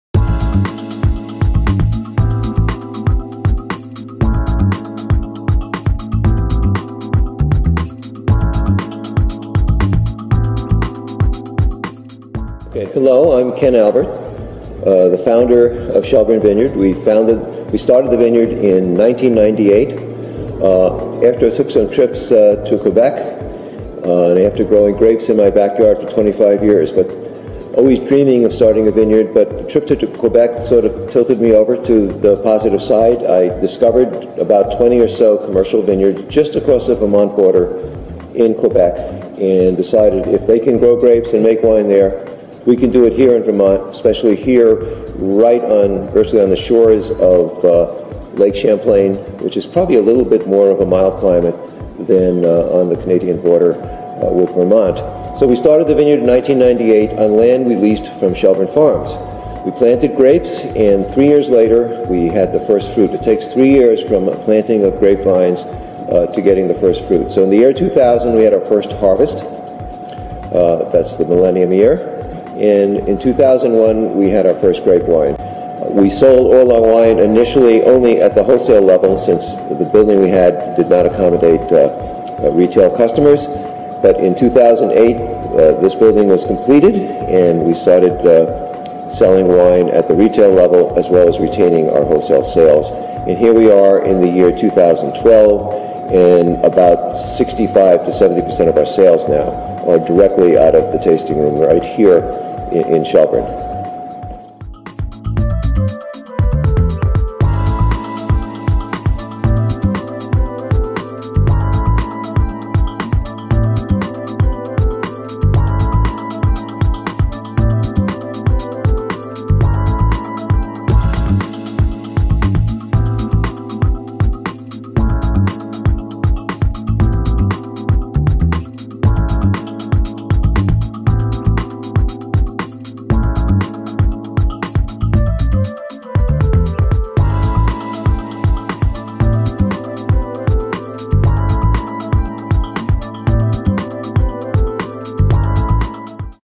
Shelburne Vineyard-Our Story
Founder and Winemaker Ken Albert welcomes you to Shelburne Vineyard, Shelburne Vermont.
Video by Lisa Arms